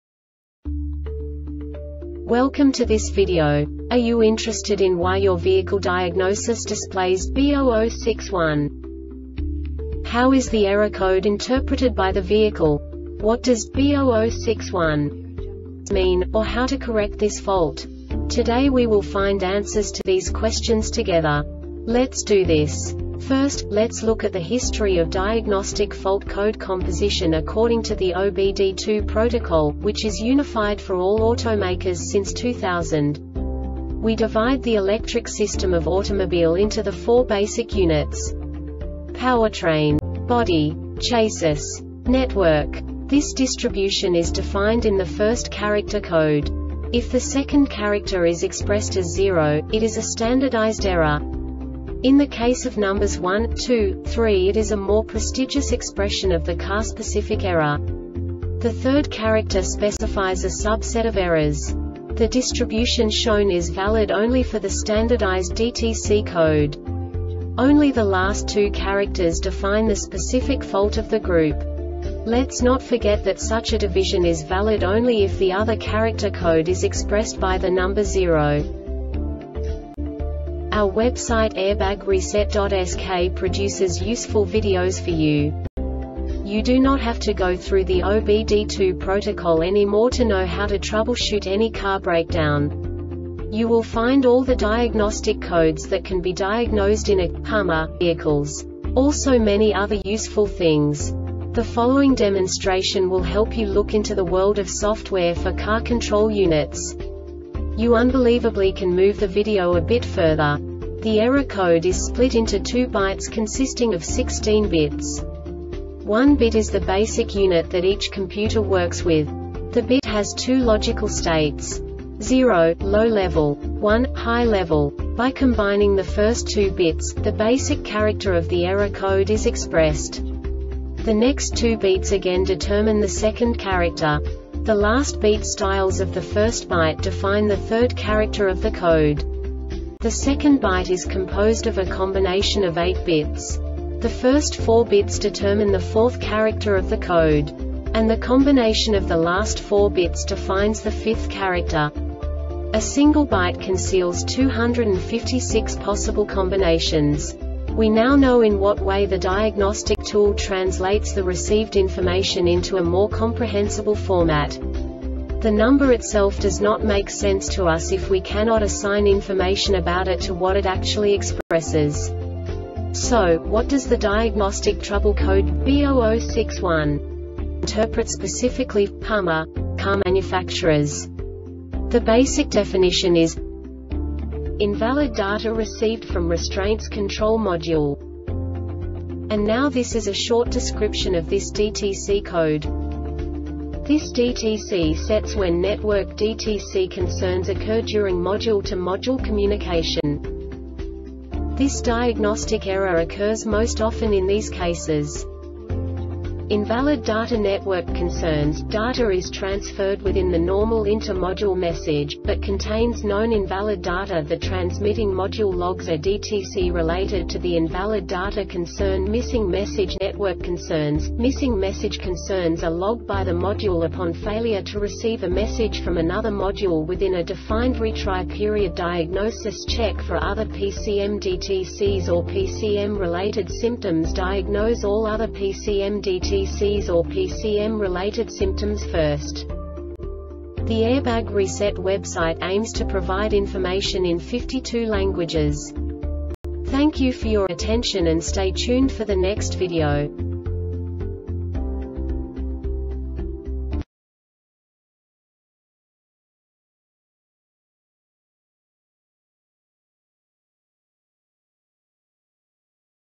DTC Hummer B0061 Short Explanation
Contents:
0:21 Basic DTC analysis according to OBD2 protocol standard.
1:48 Insight into programming
2:58 A diagnostically understandable description of the Diagnostic Trouble Code
3:05 Basic error definition

Make:
HUMMER
Code:
B0061
Definition:
Invalid Data Received From Restraints Control Module
Description:
This DTC sets when network DTC concerns occur during module to module communication.
Cause:
Invalid data network concerns - data is transferred within the normal inter-module message,  but contains known invalid data.  The transmitting module logs a DTC related to the invalid data concern.Missing message network concerns - missing message concerns are logged by the module upon failure to receive a message from another module within a defined retry period.